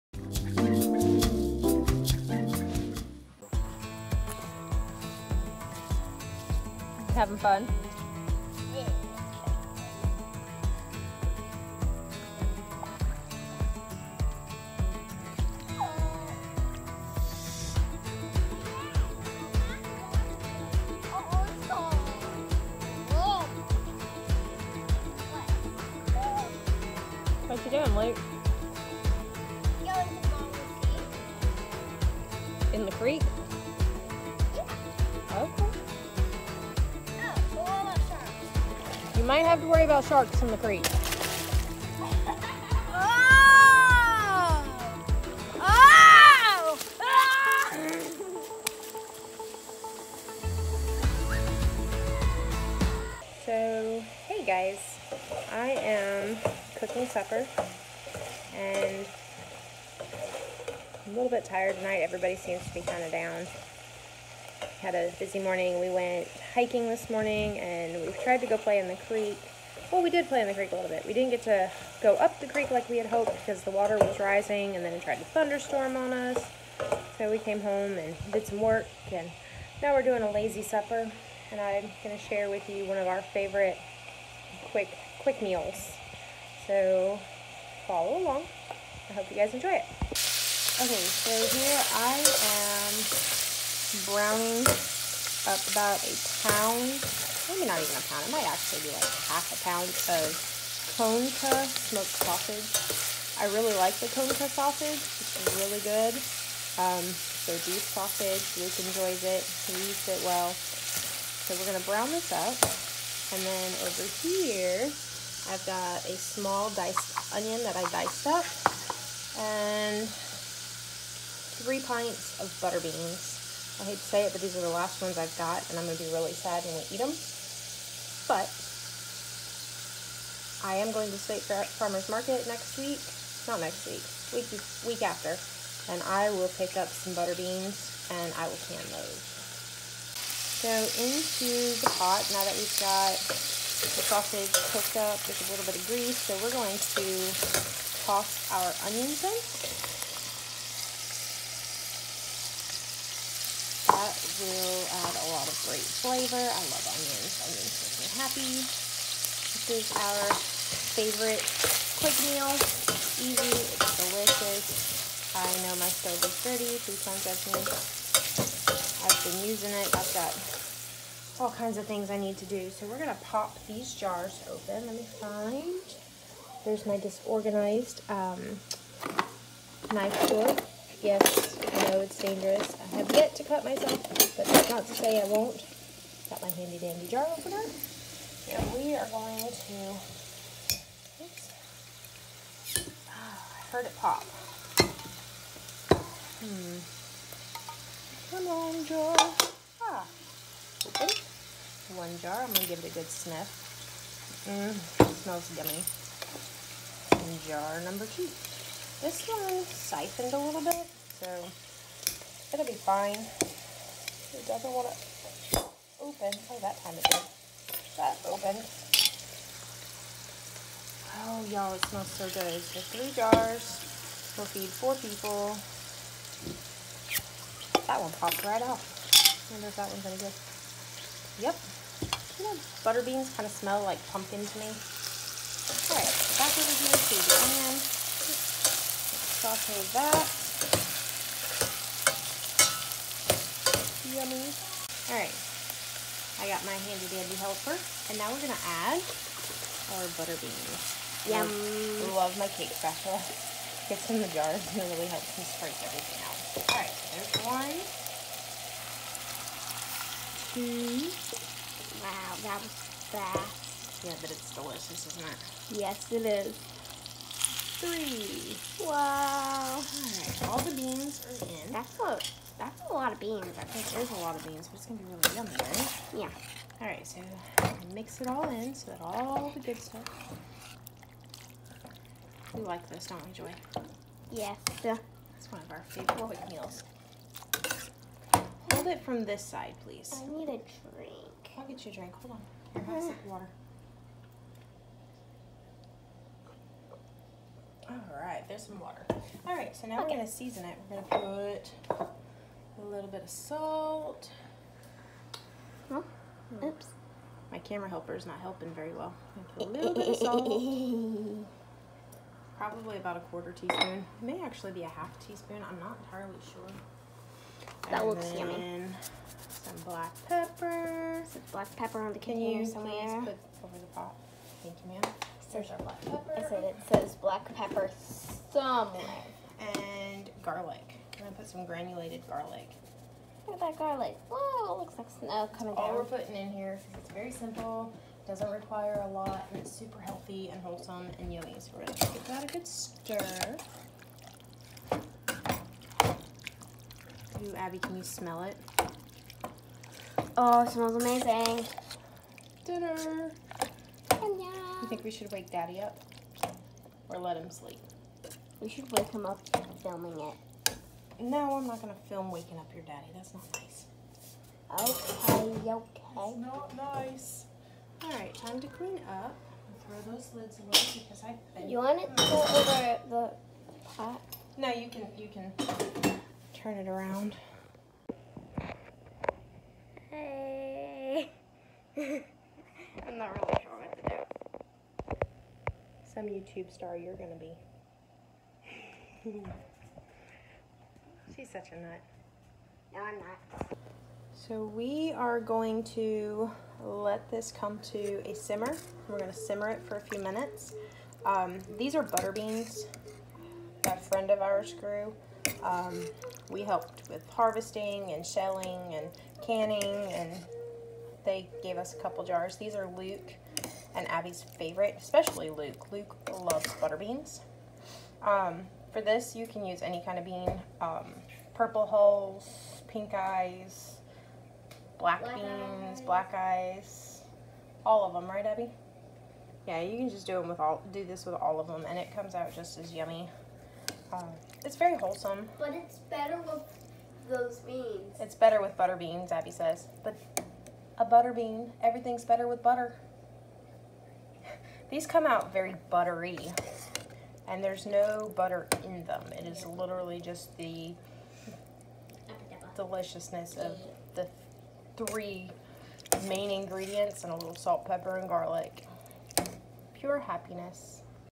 Comfort Food
Today, we will share our families favorite, simple comfort food. This recipe is super simple and so filling.
All you need is a jar or three of your favorite bean/pea, a small diced onion and about 1/2lb of your favorite smoked sausage. We used butterbeans and Conecuh brand sausage.
This meal is cheap and easy and will happily feed the whole family.